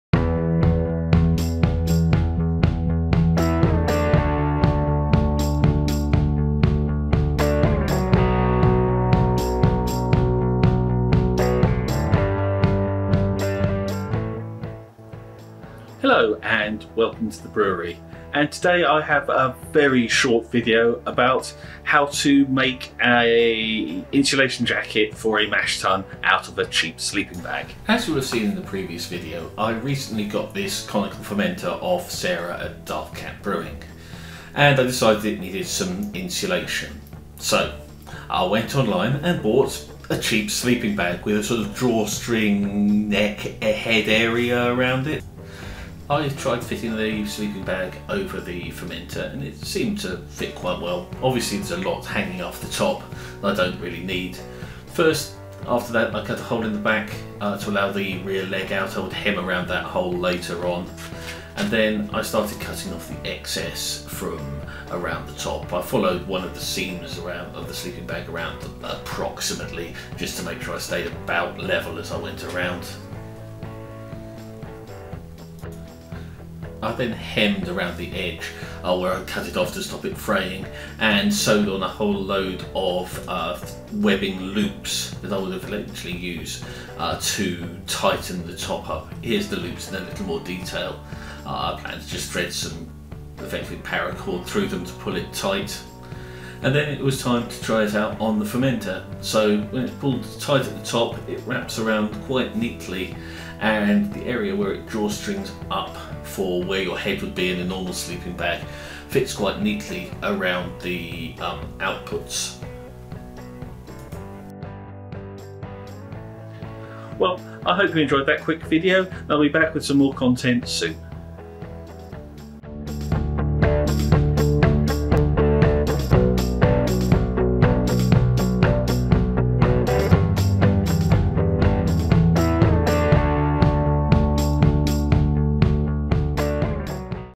Making a Fermenter Insulation Jacket from a Cheap Sleeping Bag
I needed to add some insulation to a stainless conical fermenter so tried to hack something together from a cheap sleeping bag.